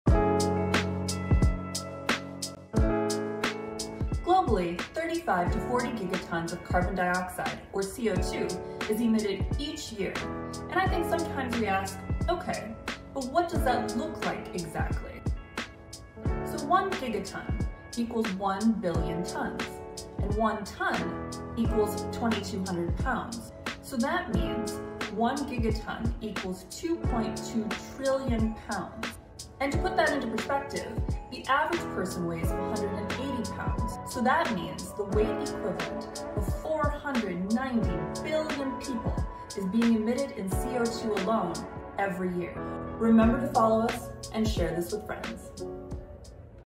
How Much Is a Gigaton (Gt)?!
🌎 Globally, 35 to 40 gigatons of carbon dioxide or CO2 is emitted each year, but what does that look like?!

👩‍🏫 Let's crunch the numbers!
⚖️ 1 gigaton = 1 billion tons
⚖️ 1 ton = 2200 lbs
⚖️ 1 gigaton = 2.2 trillion lbs
⚖️ Average person = 180 lbs

This means:
🏭 40 gigatons of annual CO2 emissions = 🧍 weight of ~490 billion people

🎞️ Watch this short reel to learn more and share with friends!